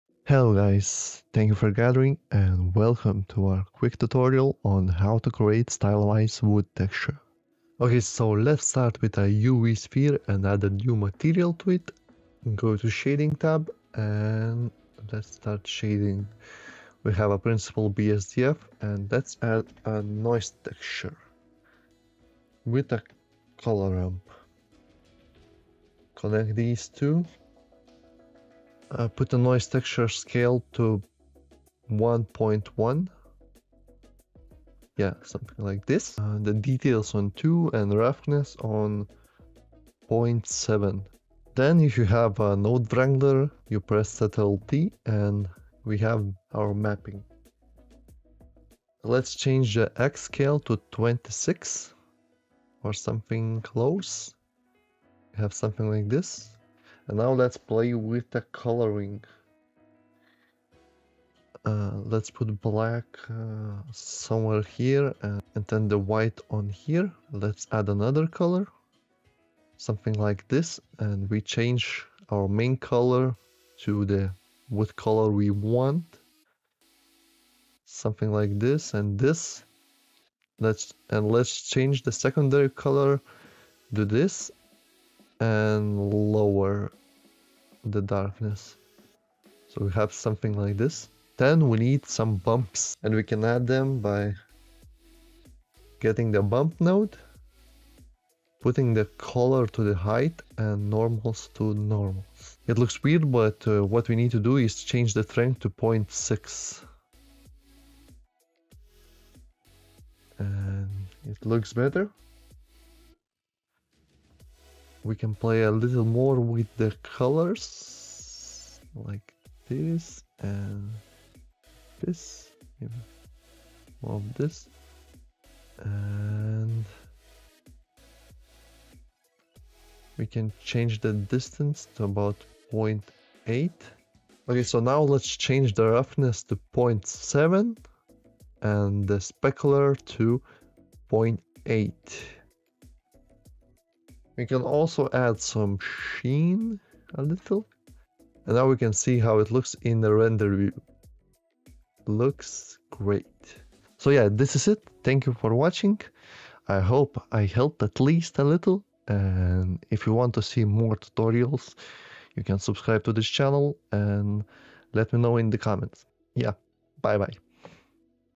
Easiest Stylized Wood In Blender
Learn how to create a stunning stylized wood material/texture in Blender with this quick and easy tutorial. This step-by-step guide covers everything from setting up the material nodes to adjusting the texture settings. Whether you're a beginner or an experienced Blender user, this tutorial will give you the knowledge you need to create professional-looking wood materials in no time.

Works on Eevee and Cycles!